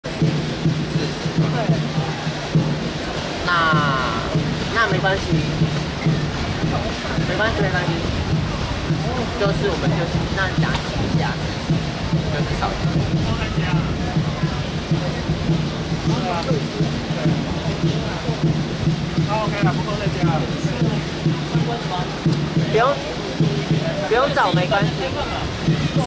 新北投車站 驛站特展 開幕 新北投車站二期工程 台鐵35TP32873三等普通客車 開放展示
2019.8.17 新北投車站 驛站特展 開幕 新北投車站二期工程 台鐵35TP32873三等普通客車 開放展示